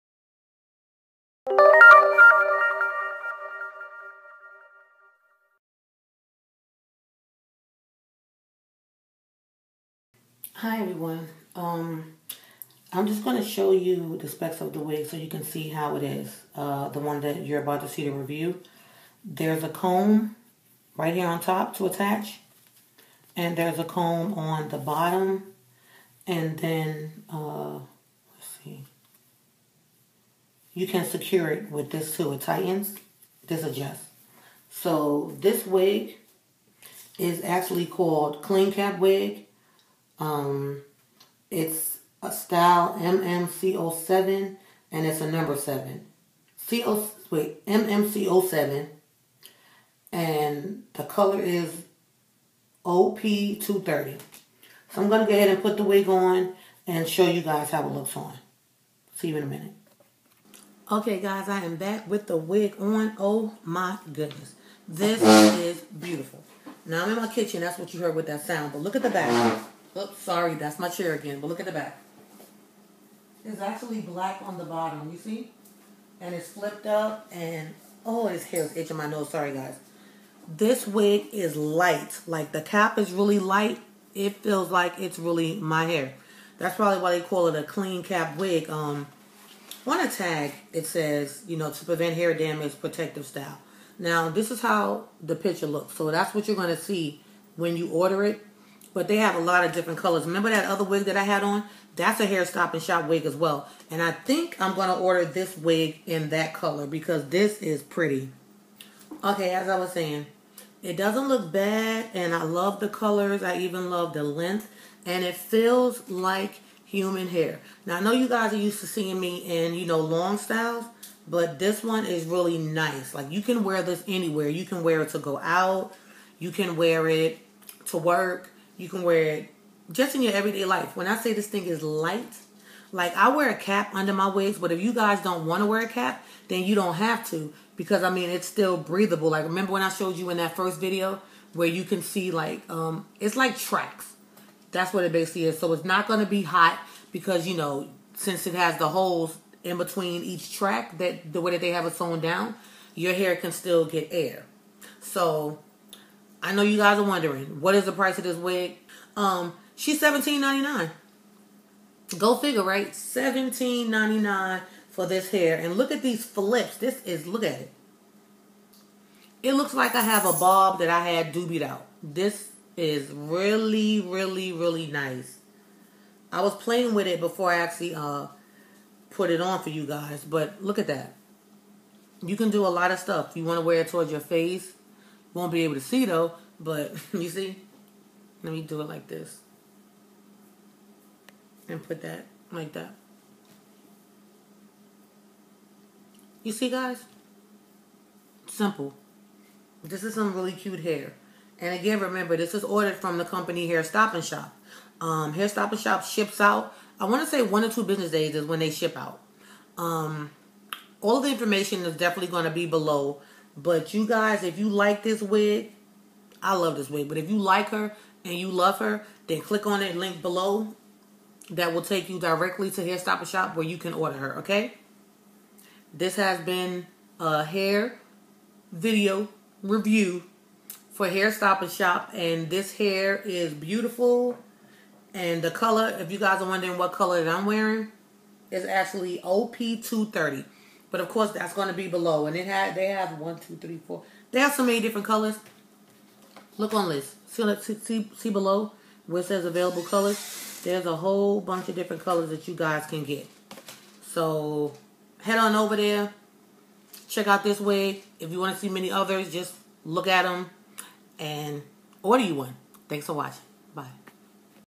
HAIR STOP AND SHOP Model Model Clean Cap WIG REVIEW
This is a wig review for the company Hair Stop And Shop

Wig Information
Name: Model Model Clean Cap Wig Number 7
Color: OP30
Length: 12"-14" inches
Price: $17.90
Hair Type: Heat Resistant Fiber Curling Iron Safe up to 400F

Available Colors:
1,1B,2,4,OP230,OP671,OP891,OP99J530130,OS1530,OS71,OS350530,OS99JPU